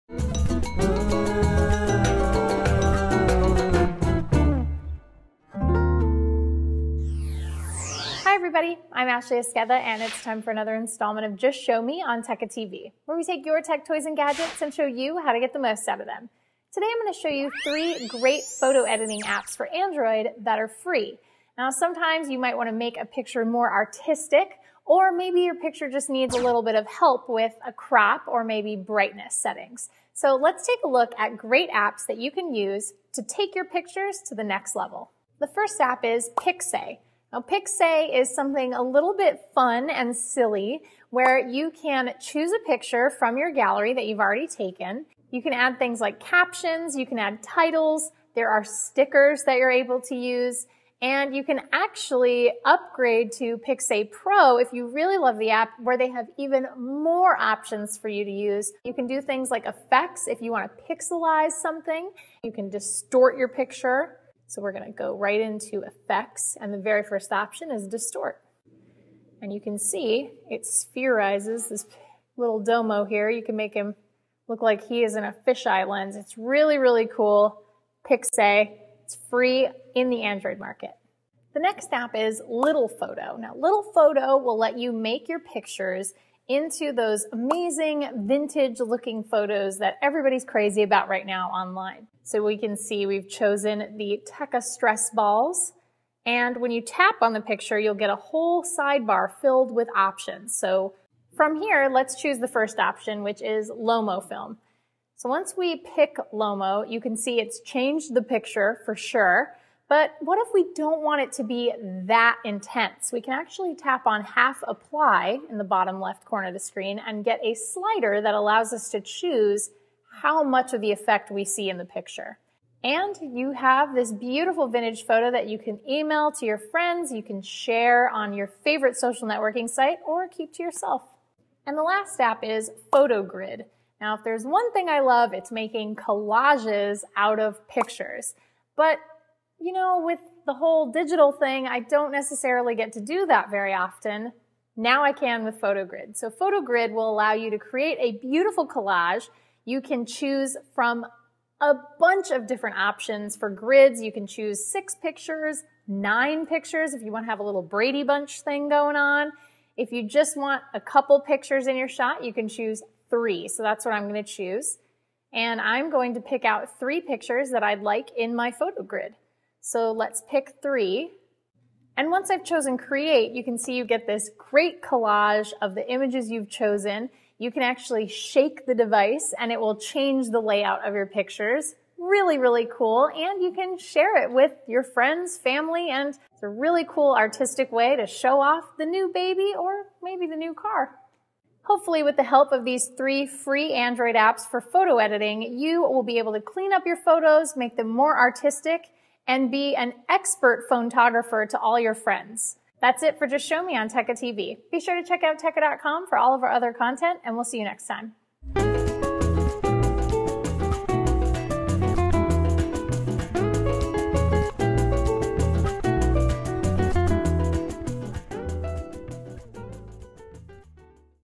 Just Show Me: 3 free photo editing apps for your Android phone
We'll show you 3 great apps to help you edit your photos right on your your Android phone. After all, your Android phone possesses an incredible camera; you should get the most out of it